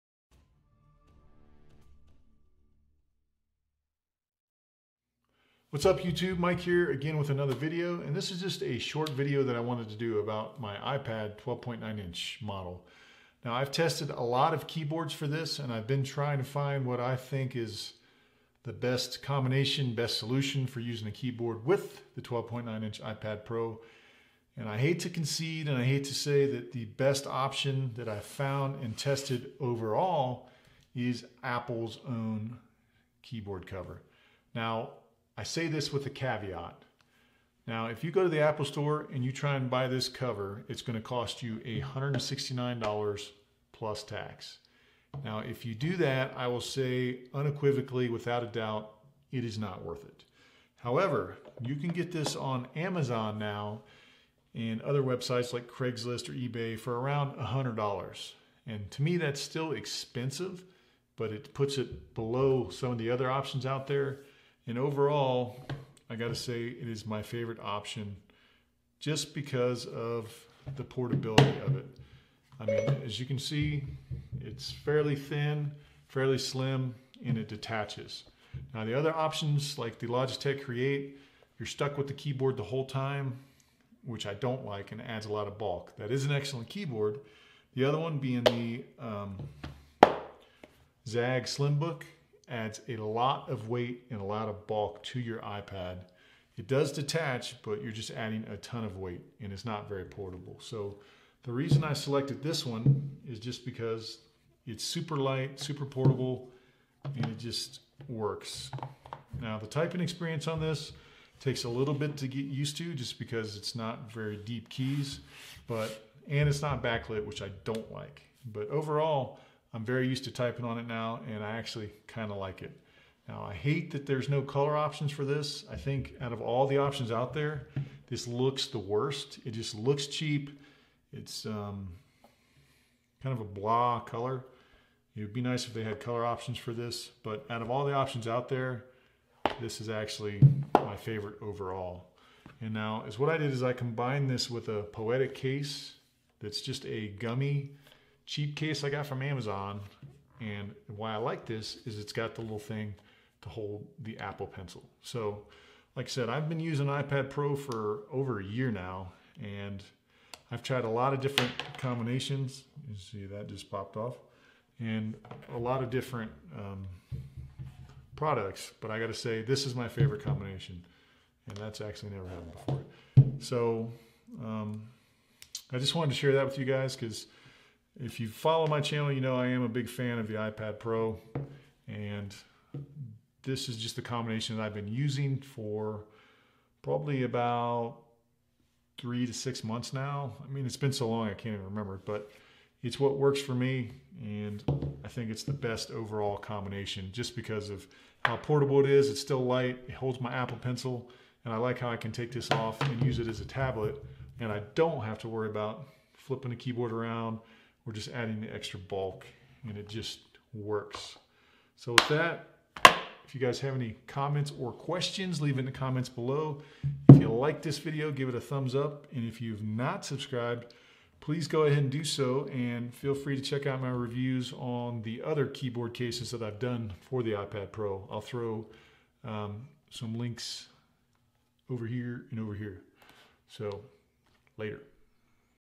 iPad Pro 12.9: My favorite Keyboard Cover, Apple's Smart Keyboard
I have tried numerous iPad Pro keyboard combos and finally found my favorite combo. Apple's own Smart Keyboard Cover.